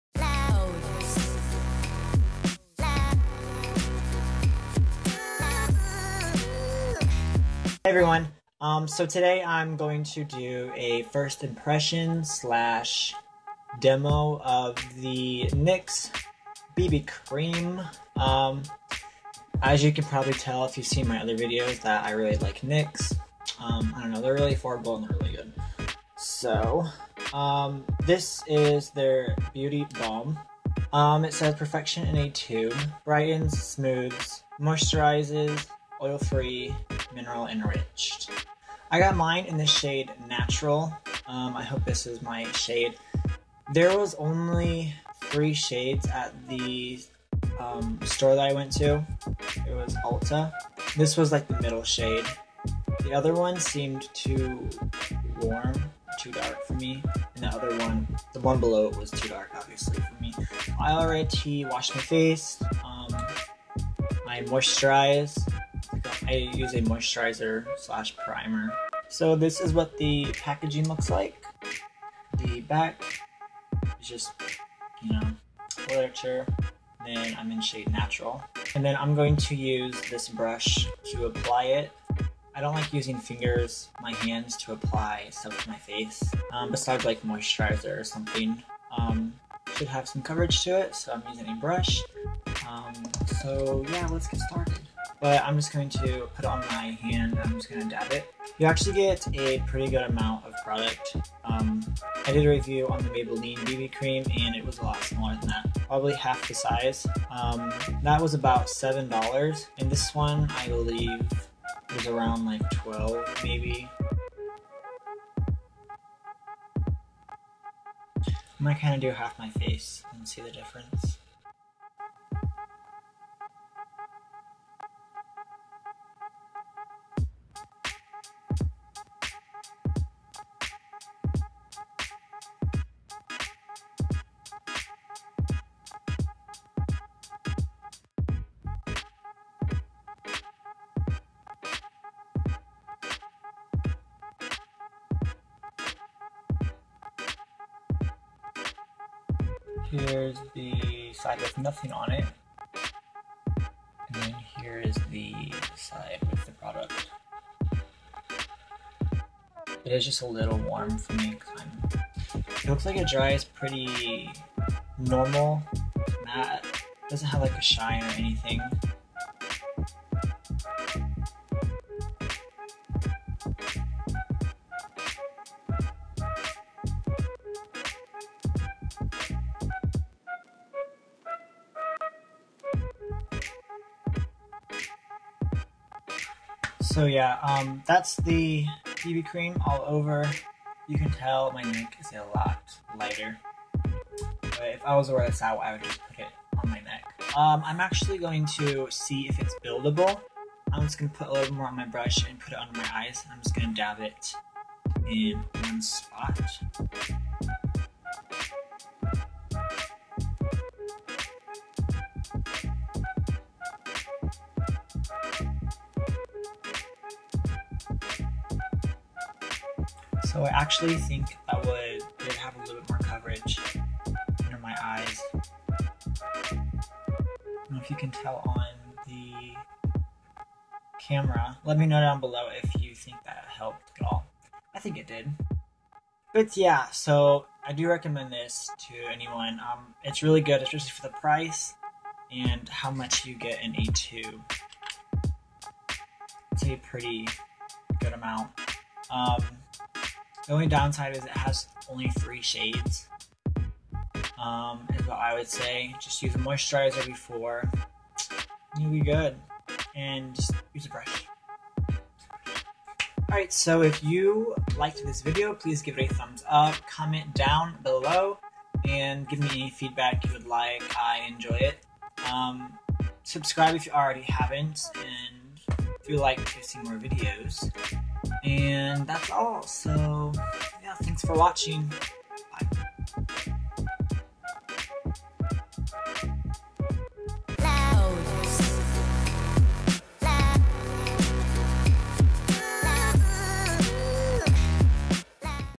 NYX BB Cream DEMO/ FIRST IMPRESSION
Hey everyone! I will be doing a demo/ first impression on the NYX BB Cream! I really love this product and recommend it to others! It looks very natural on the skin and has pretty good coverage for a bb cream. It also is great for mens makeup. They only downside is that they only come in 3 shades. I also would recommend using a moisturizer/primer before applying the product.